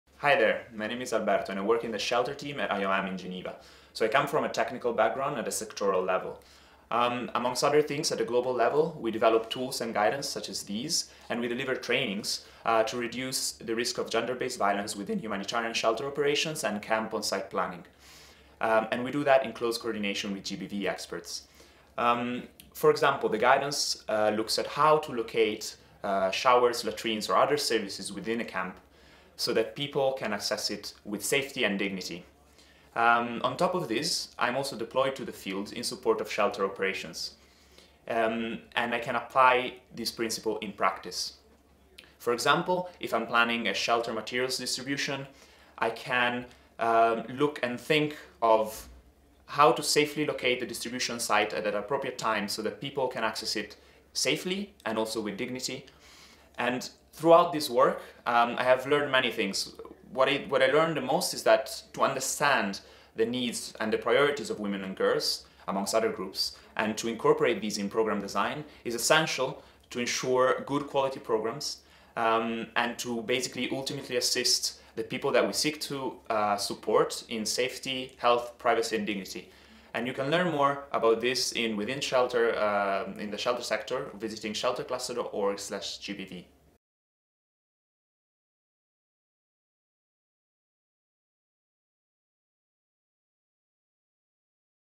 16 Days of Activism against GBV / Alberto
For the 16 Days of Activism against GBV, let us honor our colleagues’ efforts in mitigating risks, supporting survivors, and addressing root causes of GBV. Alberto Piccioli, our Shelter and Settlements Officer, explains how he addresses GBV risks to ensure good quality Shelter programming! To learn even more, see Global Shelter Cluster